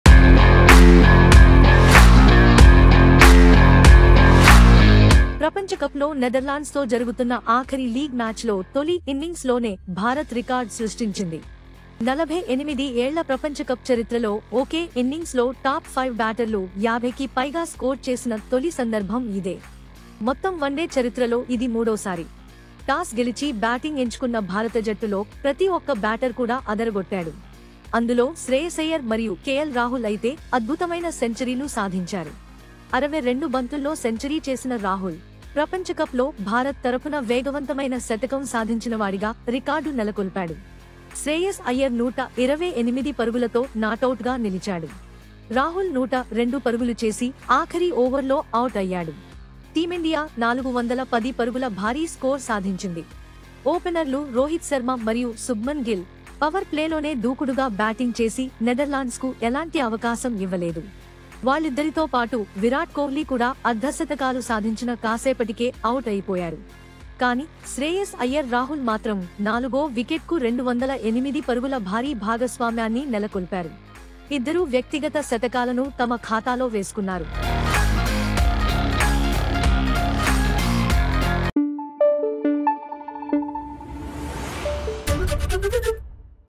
Shreyas Iyer KL Rahul Centuries | India Batting vs Netherlands: 410 స్కోర్ చేసిన భారత్ | AIra
ప్రపంచకప్ లో నెదర్లాండ్స్ తో జరుగుతున్న ఆఖరి లీగ్ మ్యాచ్ లో తొలి ఇన్నింగ్స్ లోనే భారత్ రికార్డ్ సృష్టించింది.